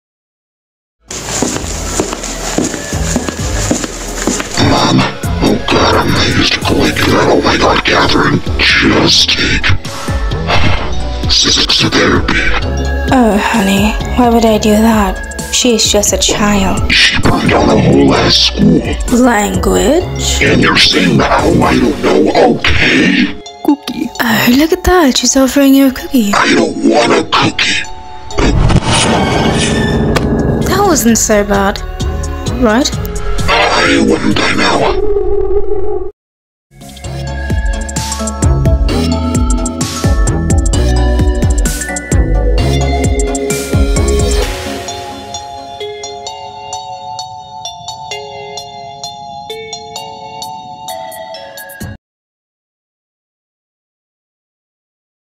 The talk and the treat By: @WiddowsMelon, And @KathrineCALBROWNFt: Shadowfreak Kathrine and six)
Comic by: Widdows and Kathrine_gaming1

Voice actors: Leoangru102, Widdows, and Kathrine_gaming1♡(Gaming gang♡

editing by: Leoangru102

Characters by:
@ShadowFreakOfficial by Funtime freddy in Discord
Kathrine calbrown by @KathrineCALBROWN
Six From Little Nightmares by Tarsier Studios
also @WiddowsMelon is in this comic thanks to her she made this comic as well